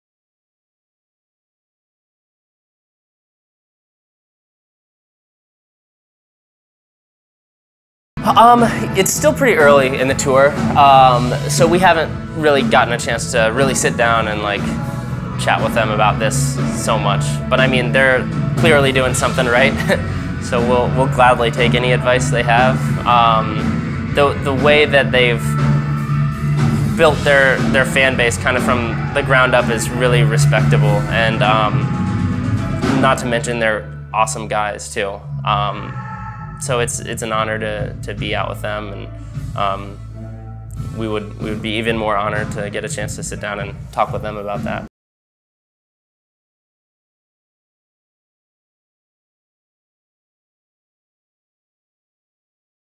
Young Rising Sons - Advice from The 1975 - Real Feels
Before their first ever show in Detroit, Young Rising Sons sat down to talk with Real Feels about their upcoming album, the music industry, touring with The 1975 and more.